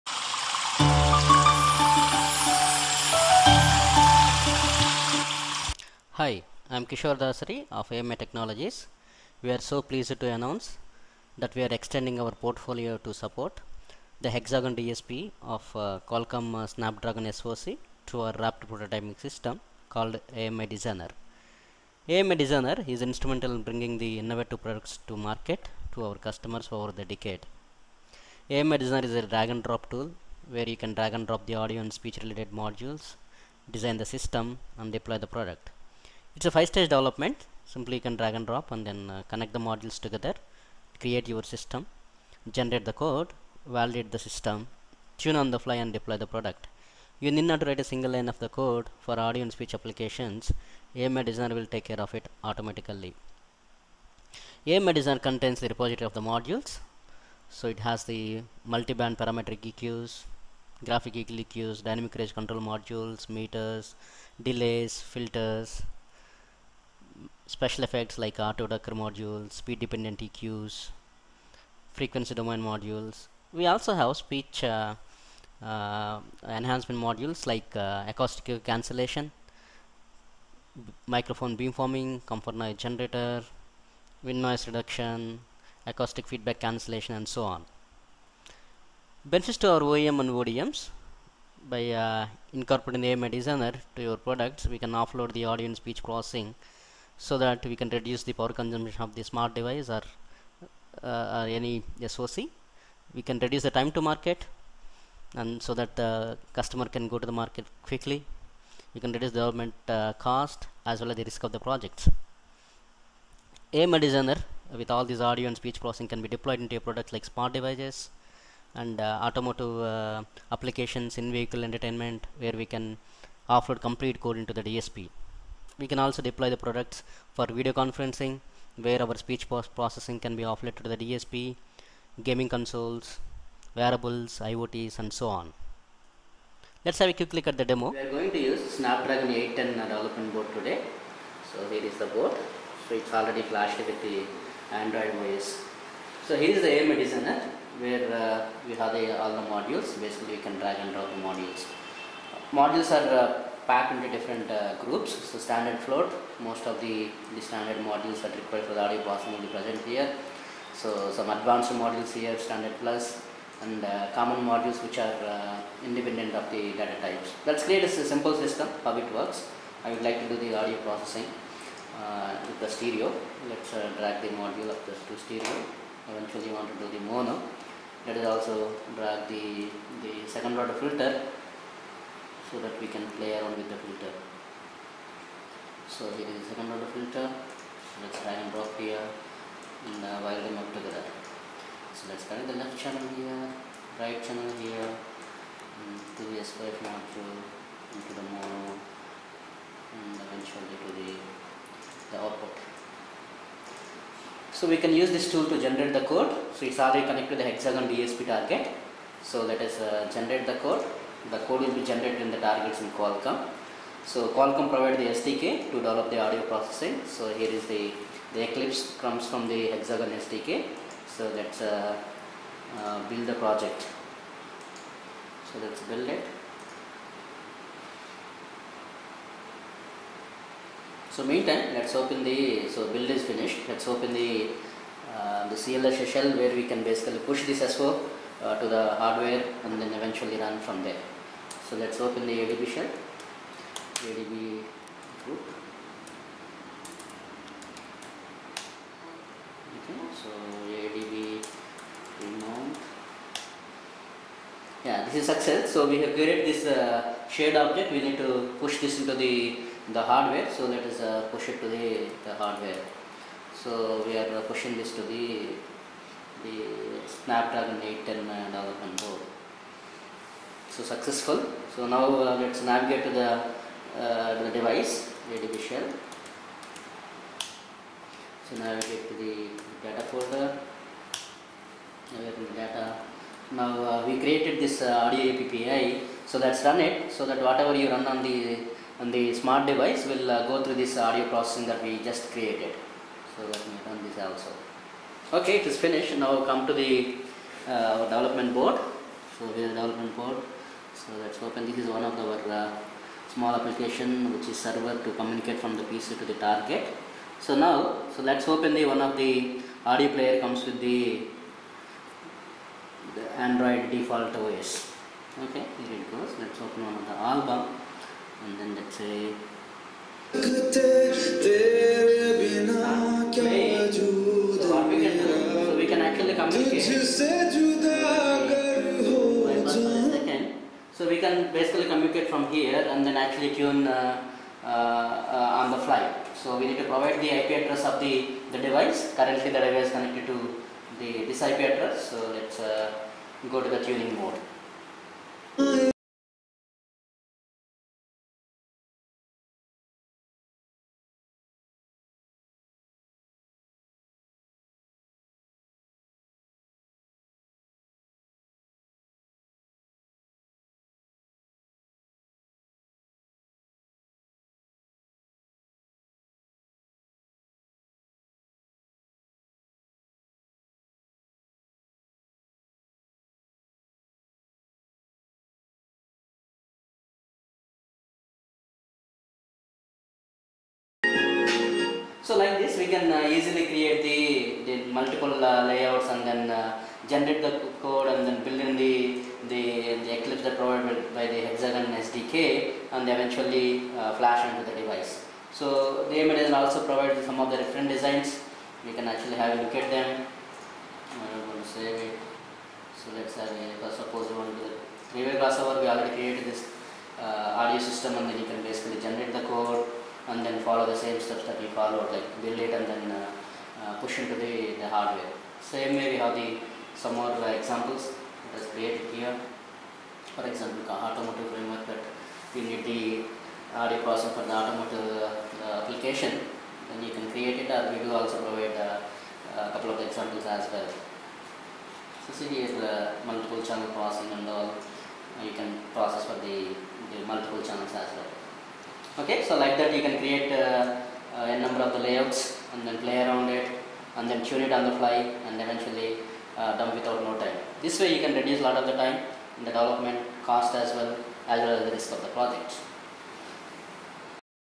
Accelerating Audio and Speech developments on Hexagon DSP, Qualcomm Snapdragon Processors
This Video demonstrates Accelerating Audio And Speech Processing developments on Hexagon DSP by Drag and Drop DSP modules Tool QualComm Snapdragon Processors. Simply Offloading the Signal processing and Audio Post Processing on to Hexagon DSP with simple drag and drop tool by AMI Technologies.